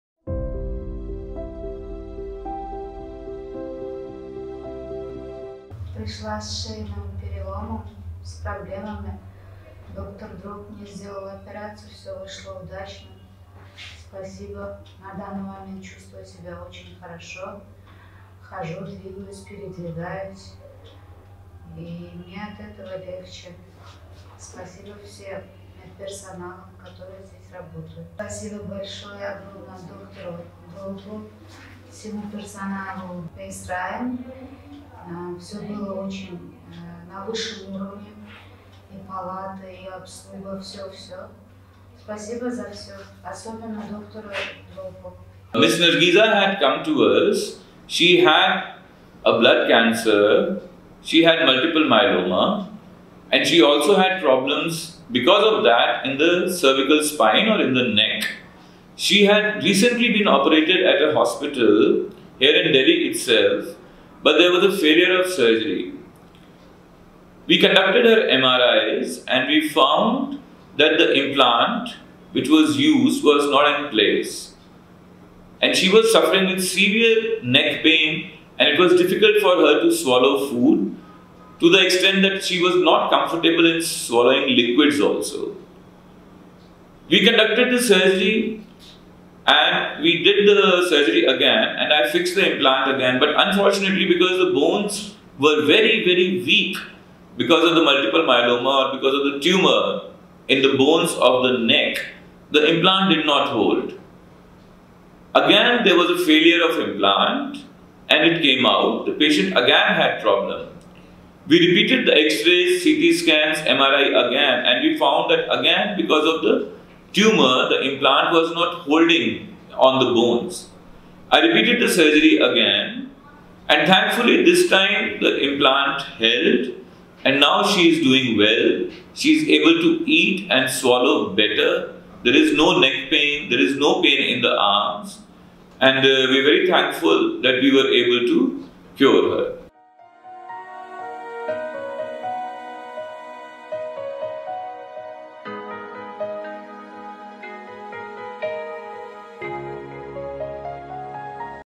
Happy Patient Testimonial | Dr. Dhruv Chaturvedi, PSRI Hospital Delhi NCR
Nargisa underwent surgery for a tumor and got neck implants in Delhi recently but the results weren't satisfactory as the implants used were not placed correctly. She came to PSRI Hospital with problems in the neck and spinal cord, where she underwent cervical implant surgery under Dr. Dhruv Chaturvedi. Nargisa is on the road to recovery under the care of expert neuro doctors and will soon be able to return to her normal life. Hear what Dr. Dhruv has to say about the surgery and how he completed the procedure.